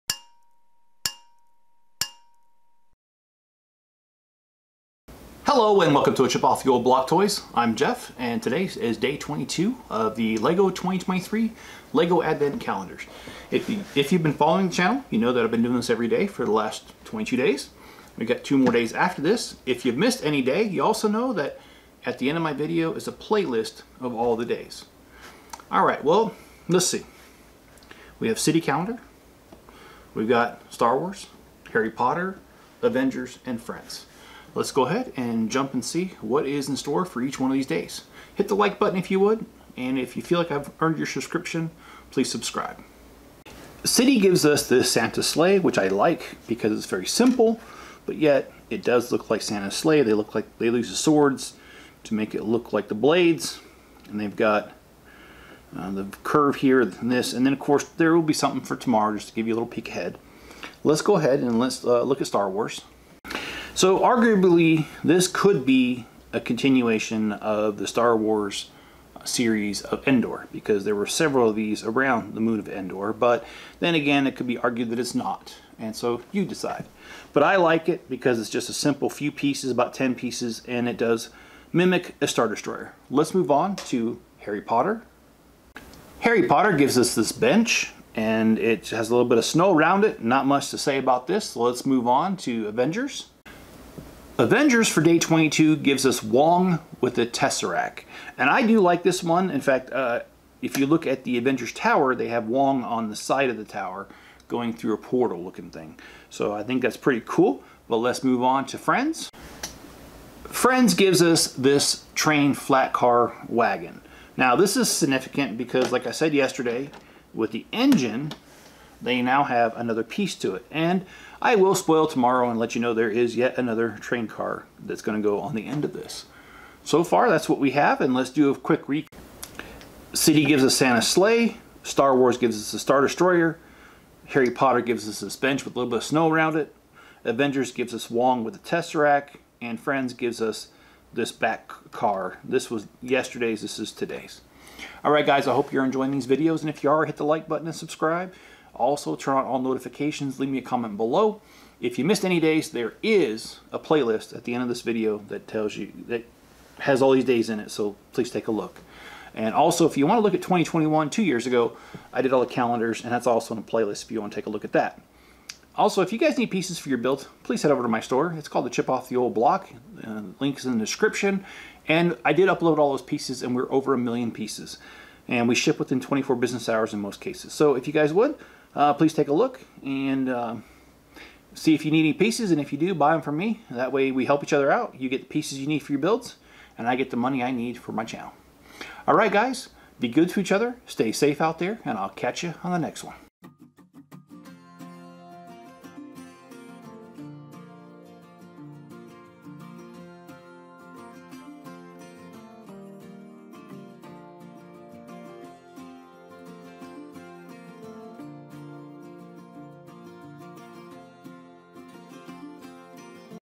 Lego 2023 Advent Calendars Day 22
Join us each day now and until Christmas to see what inside each day of the Lego Advent calendars of 2023.  We have all five calendars sets this year.  60381 City Advent Calendar 2023, 75366 Star Wars Advent Calendar 2023, 76418 Harry Potter Advent Calendar 2023, 76267 Avengers Advent Calendar 2023, and 41758 Friends Advent Calendar 2023.

Day 22:
City-Santa Sleigh
Star Wars-Imperial Star Destroyer
Harry Potter-Bench
Avengers-Wong with Tesseract
Friends-Train Flat Car/Wagon